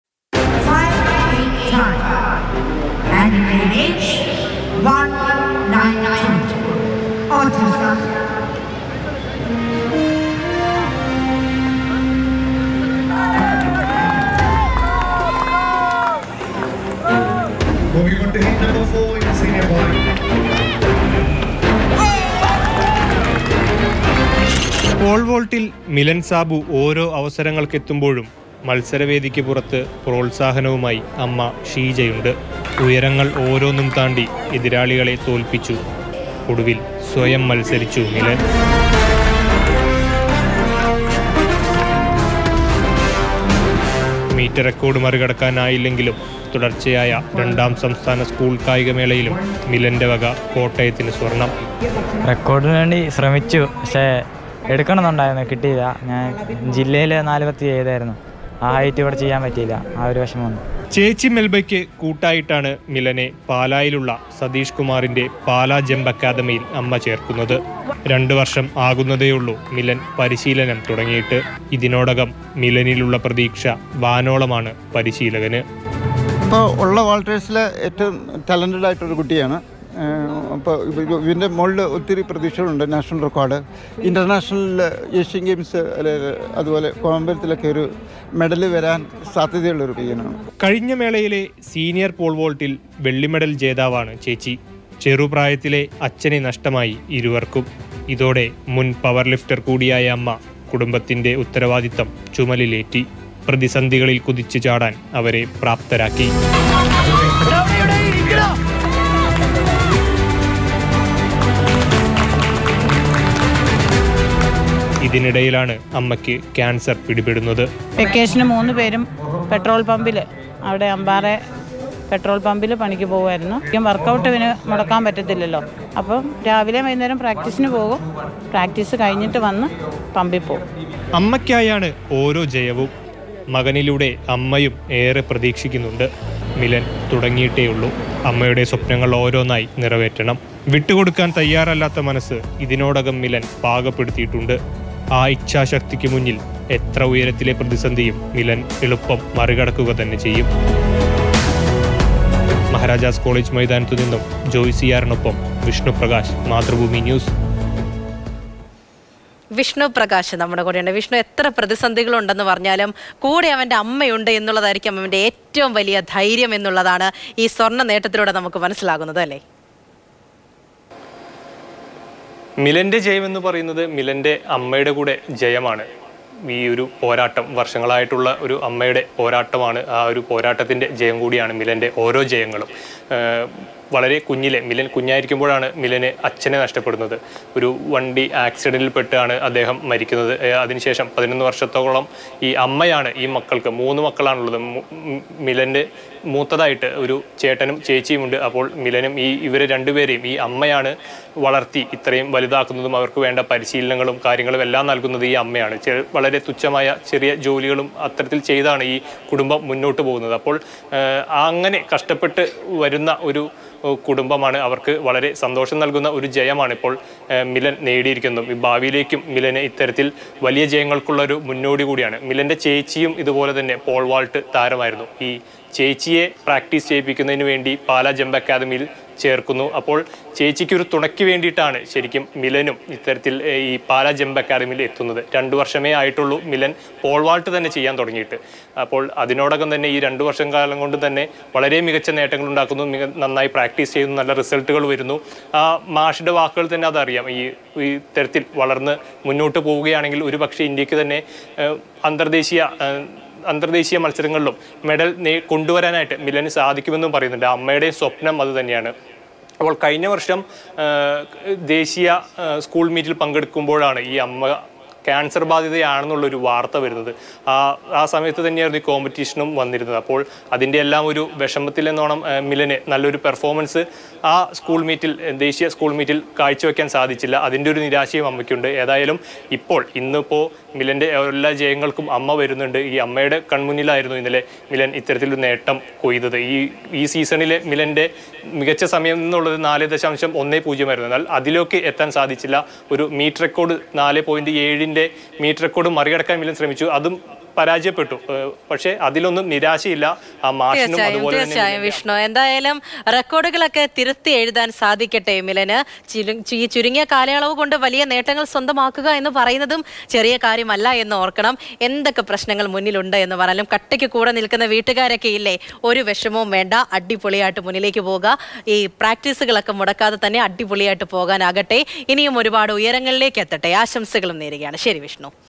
മകനേ... മകനേ വാ... ജീവിത പ്രയാസങ്ങള്‍ക്ക് മേല്‍ മിലന്റെ കുതിച്ചുചാട്ടം
കാൻസർ ബാധിതയായ അമ്മ, അച്ഛന്റെ മരണം, തിരിച്ചടികളിൽ പതറാതെ മിലൻ സാബു.. അഭിമാനമായി പോൾ വോൾട്ട് ജൂനിയർ വിഭാഗത്തിൽ സ്വർണം നേടിയ കൊച്ചുമിടുക്കൻ
മാറുന്ന ലോകത്ത് കൂടുതൽ മാറ്റങ്ങളോടെ. പുതിയ വേഗത്തിൽ. പുതിയ ലുക്കിൽ.
മാതൃഭൂമി ന്യൂസ്.

Happy viewing!